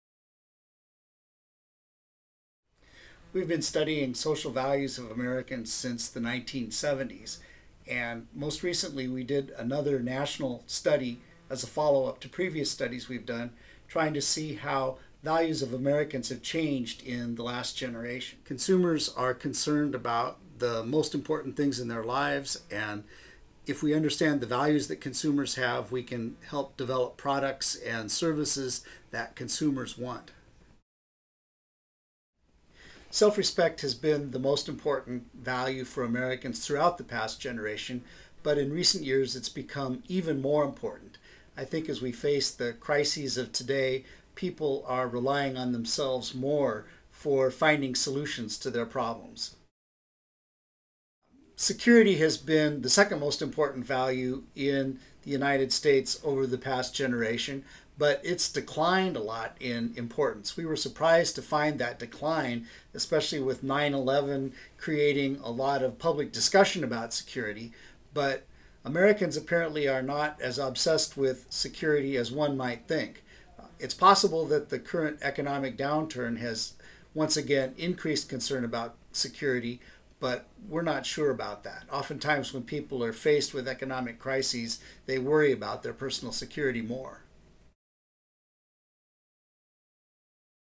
SocialValues2007.m4v
University of Oregon marketing researchers have found that self-respect in 2007 has jumped higher in importance as a social value among Americans than in two previous surveys (1976 and 1986). Surprisingly, they found security has dropped dramatically as a desired social value. In this video, Lynn R. Kahle, professor of marketing, gives an overview of the study, which was published in the Journal of Advertising Research and led by his doctoral student Eda Gurel-Atay.